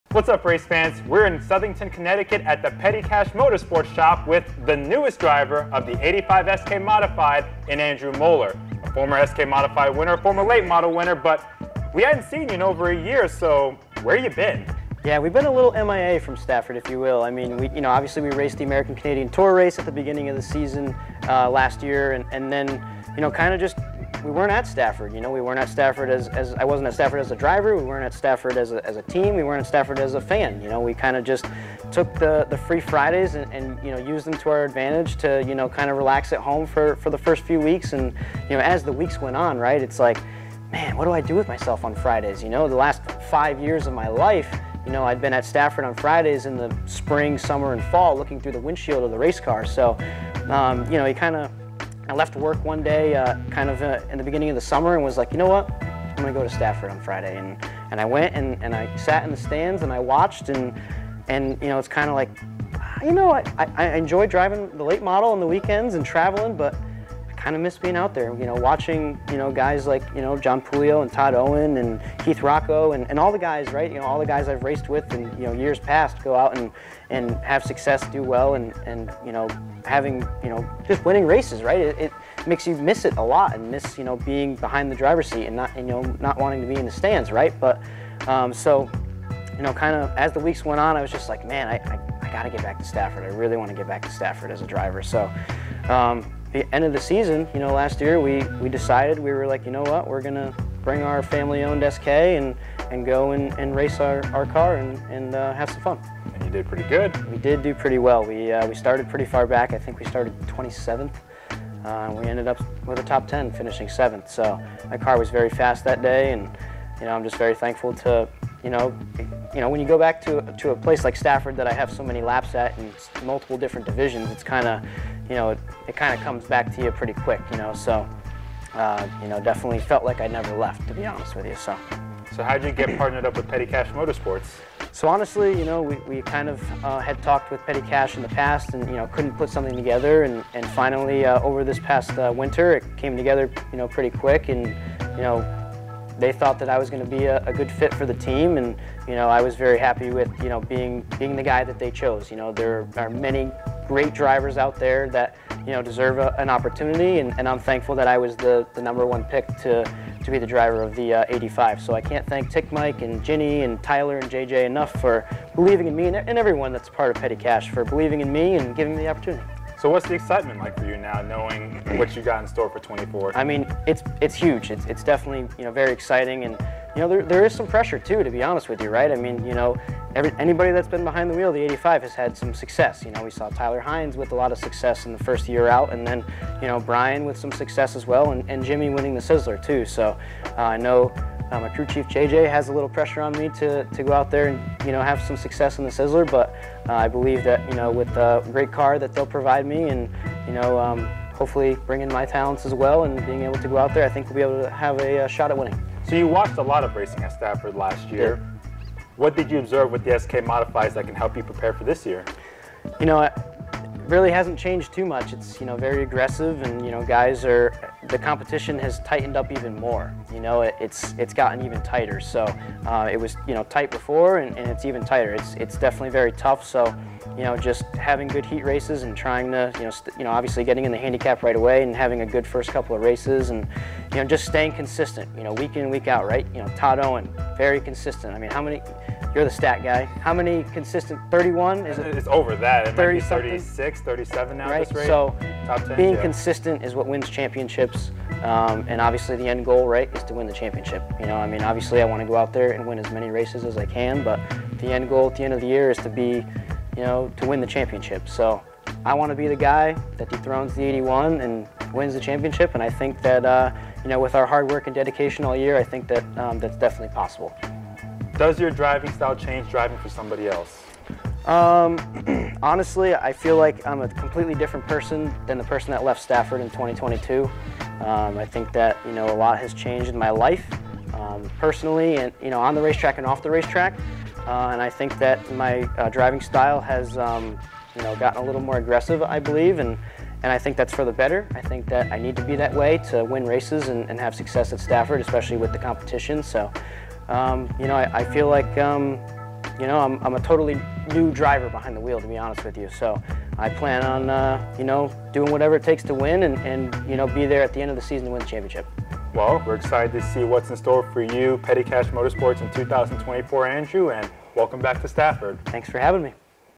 Andrew Molleur on Joining Petty Cash Motorsports
Andrew Molleur sat down with Bonssa Tufa to talk about returning to Stafford weekly, how he got teamed up with Petty Cash Motorsports, and what the outlook is for 2024.

Molleur has won at Stafford in the SK Light Modifieds, Late Models, and SK Modifieds.